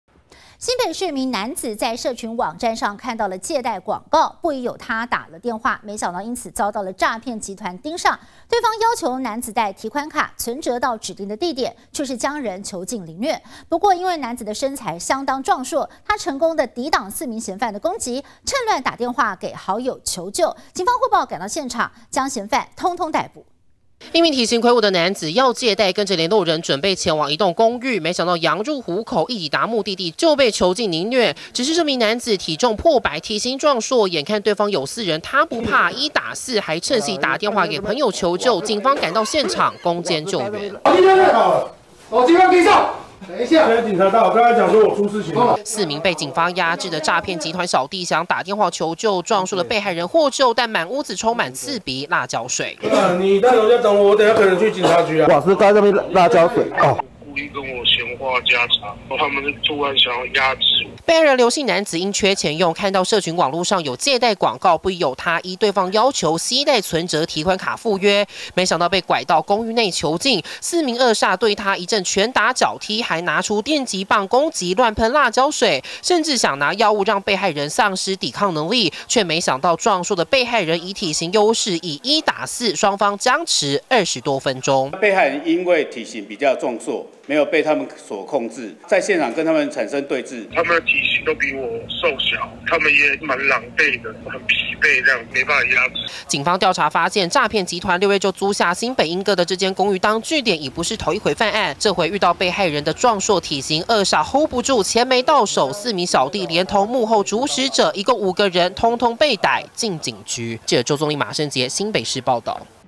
想借貸遭囚禁凌虐 壯男一打四趁隙求救
新北市一名男子，在社群網站上看到借貸廣告，不疑有他打了電話，結果遭詐騙集團盯上。對方要求男子帶提款卡、存摺到指定地點，卻是將人囚禁凌虐！不過，因為男子身材相當壯碩，成功抵擋四名嫌犯的攻擊，並趁亂打電話給好友求救，警方獲報趕到現場，將嫌犯通通逮捕。